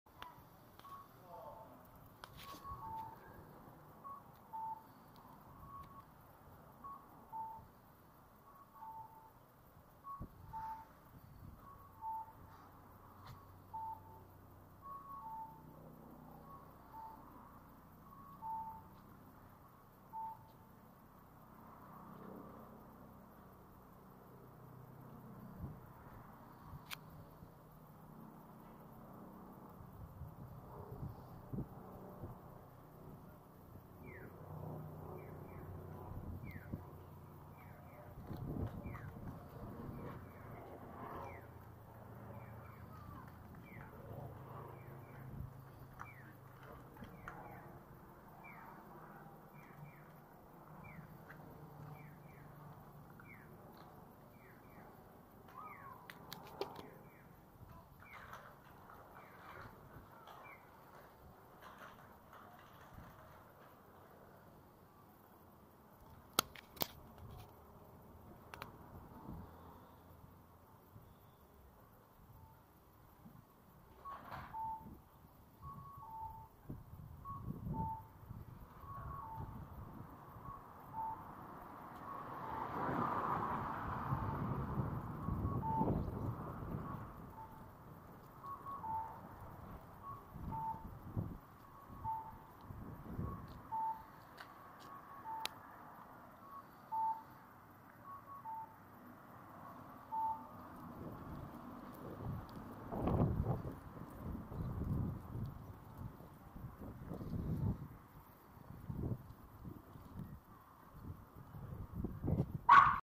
珍!? 窮屈な場所に設置された『U形歩行者用交通信号灯器』(2) / 広島県福山市
とても窮屈な場所にある『U形歩行者用交通信号灯器』
　　場所的にテント下の設置となったようですが、さて、信号機が先か？テントが先か？
　　詳細はわかりませんが、なんとも面白い設置状況でした

　　　　　　　U形歩行者用交通信号灯器
　　　　　　　仕様書番号　警交規 第219号 「版1」
　　　　　　　形式 : 人形
　　　　　　　製造番号 : Z62724
　　　　　　　製造年月 : 2000年3月
　　　　　　　㈱京三製作所

　　　　　広島県福山市松永町
　　　　　FIMI PALM　4K (3840x2160  30p)　　2021/10/17